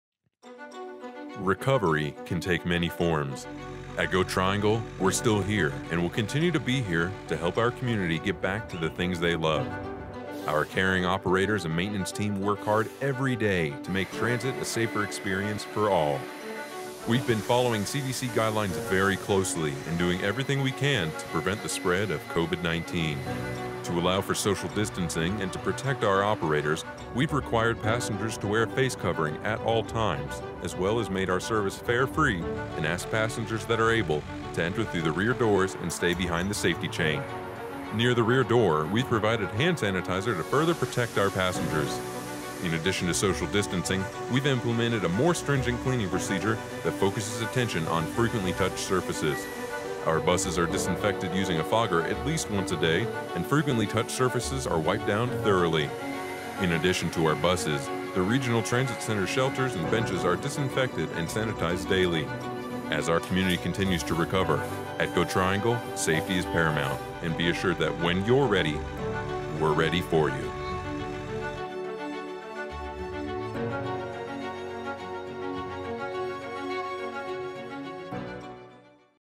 GoTriangle Covid 19 Response
SAFETY FIRST:  We wanted to remind our wonderful customers about all of the things we're doing to minimize COVID-19 risks. Please do your part as well. We're all in this together.